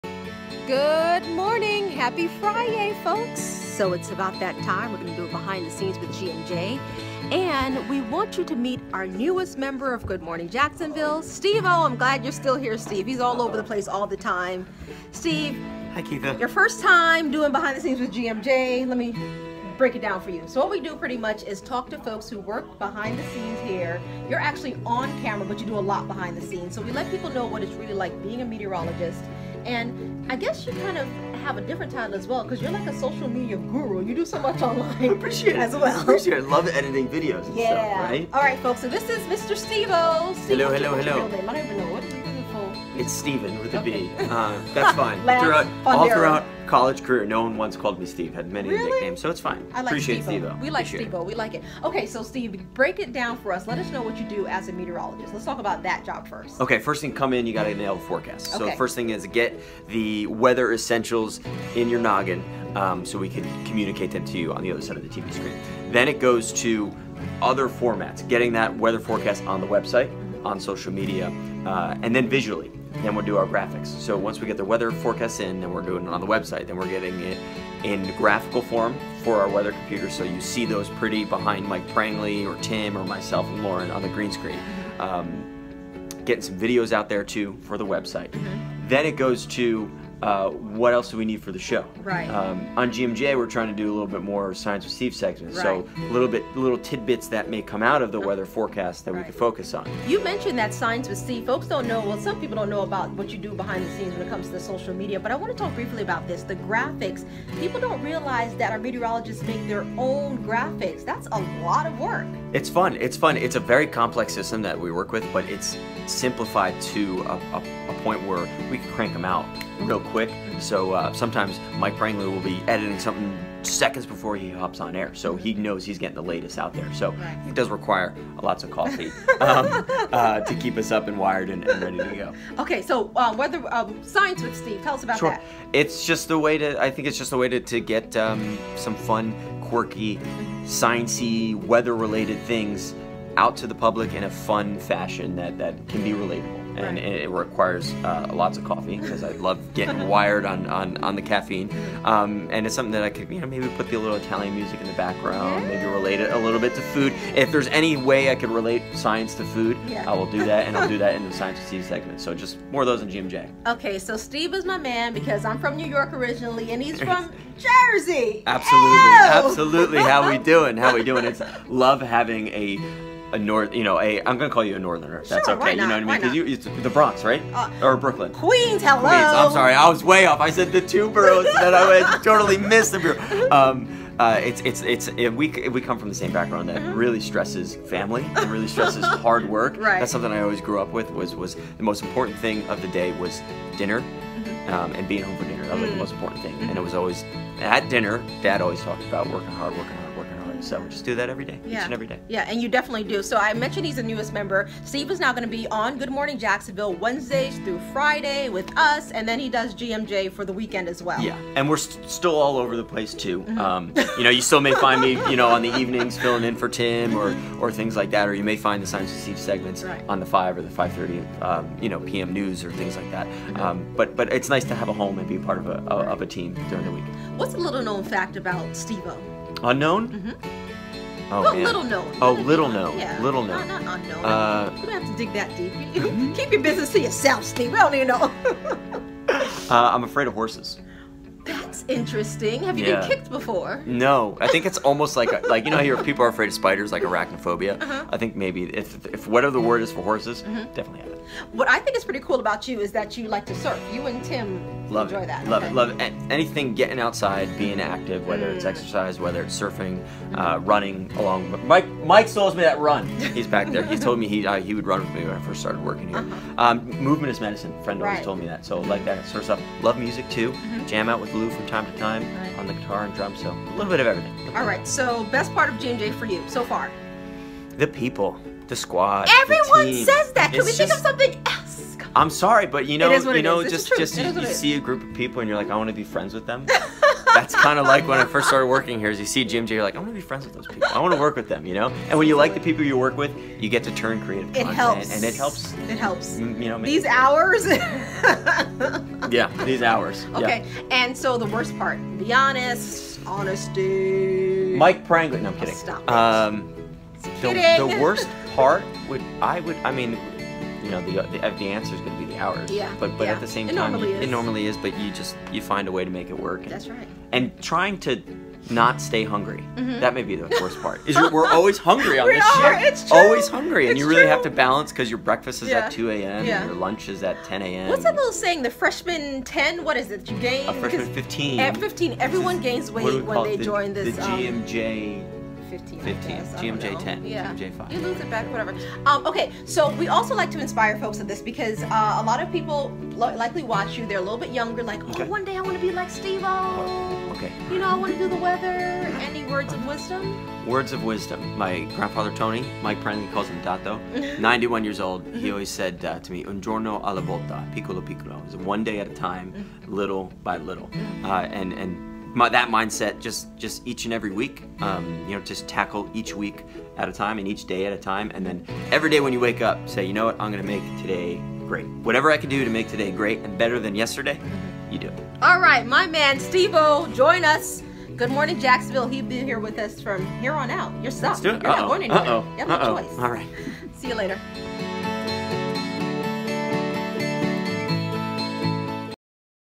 'Behind the Scenes' with GMJ
Steve Fundaro has been with First Coast News for three years. Keitha Nelson is chatting with him about his history in this episode of 'Behind the Scenes' with GMJ.